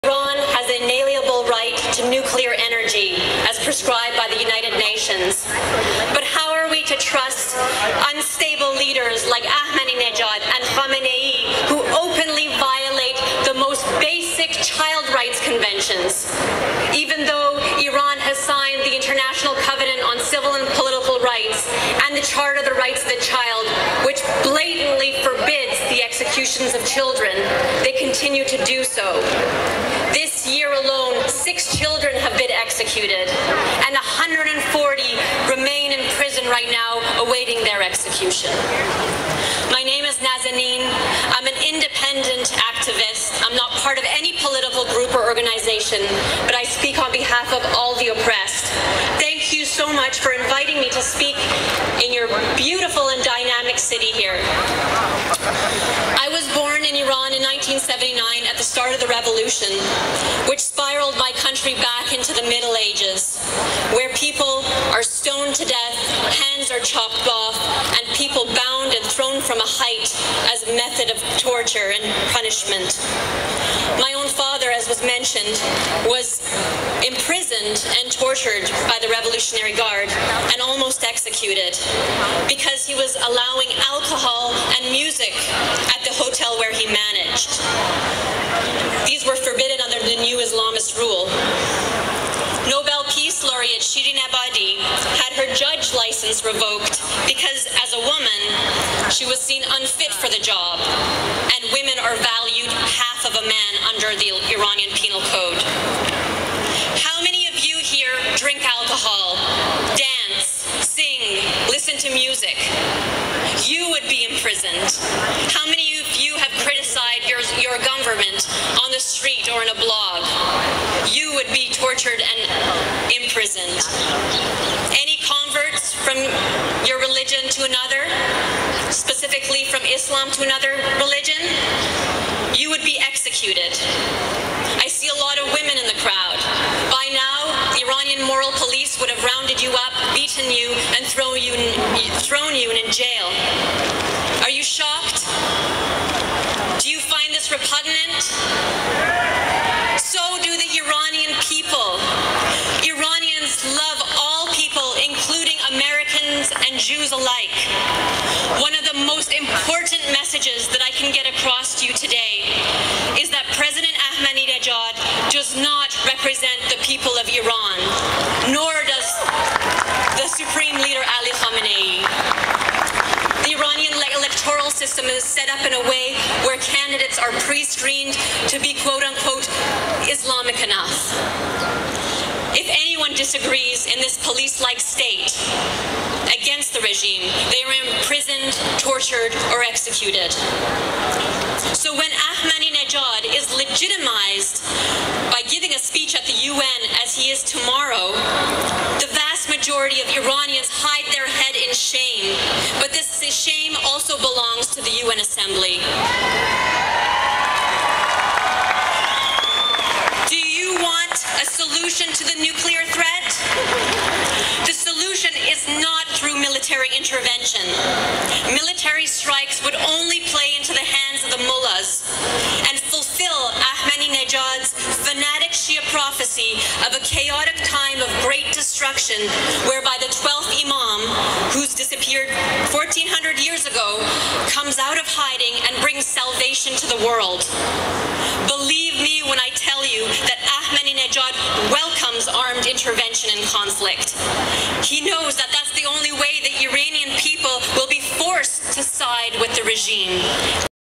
Nazanin Afshin-Jam on Human Rights in Iran
Nazanin Afshin-Jam talking on human rights in Iran during the rally 'Stop Iran Now' in front of the UN - 22 Sep 2008